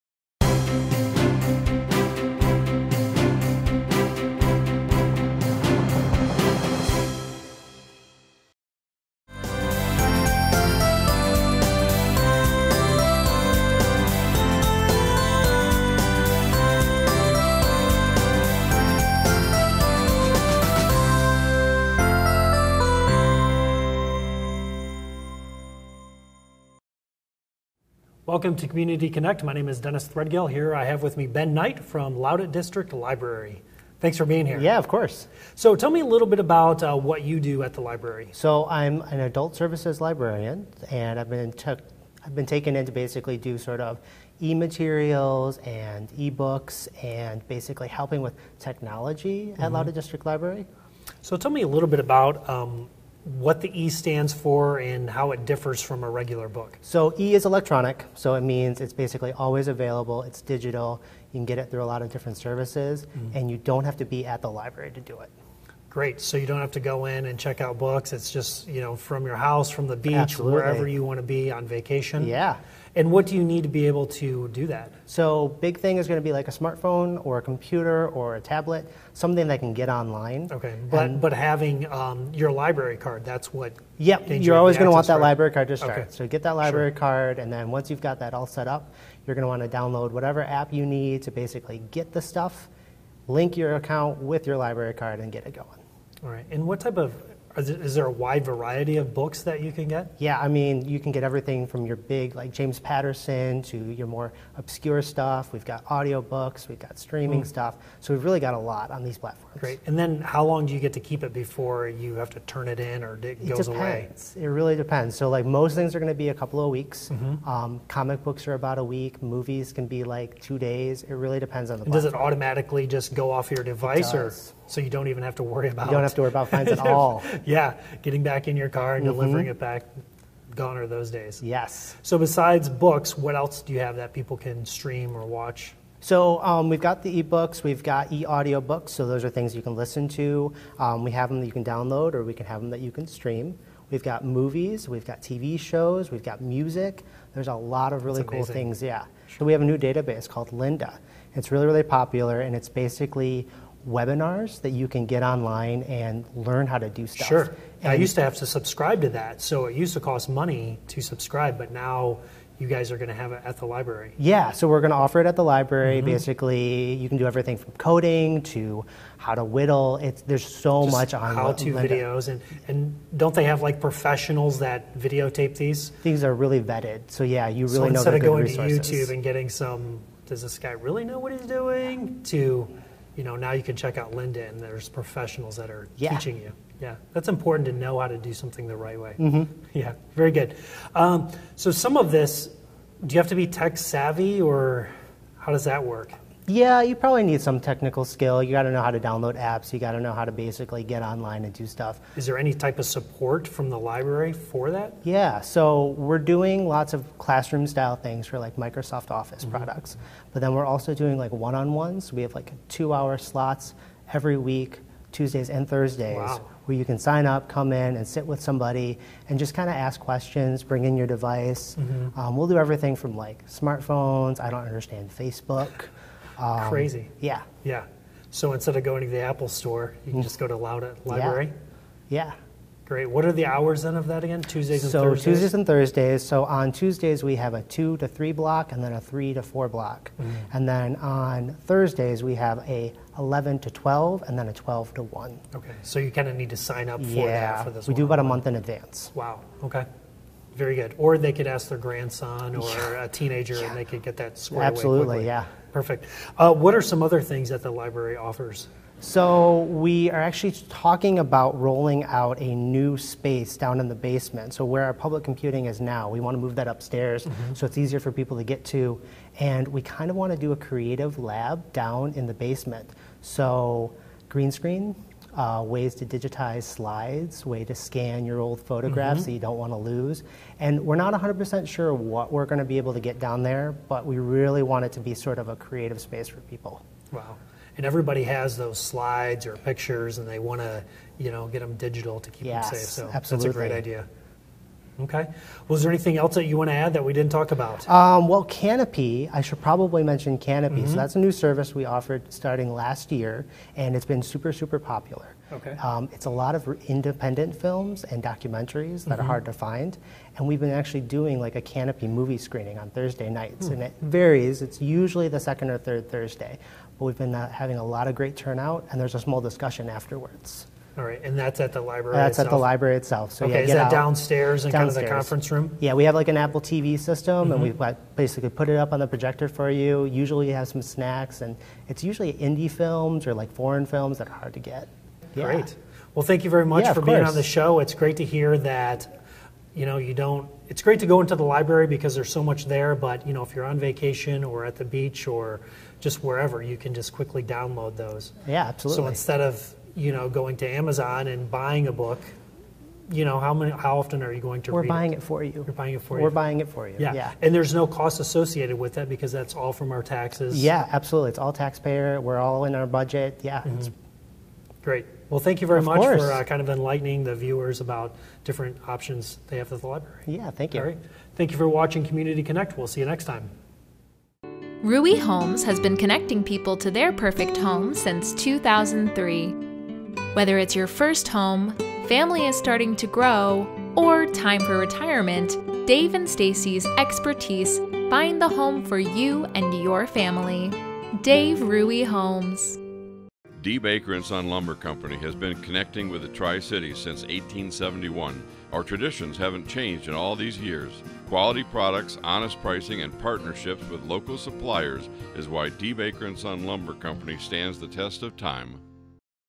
CC Ben Knight
Adult Services Librarian, Ben Knight talks about all the free online services that Loutit District Library offers to library card holders.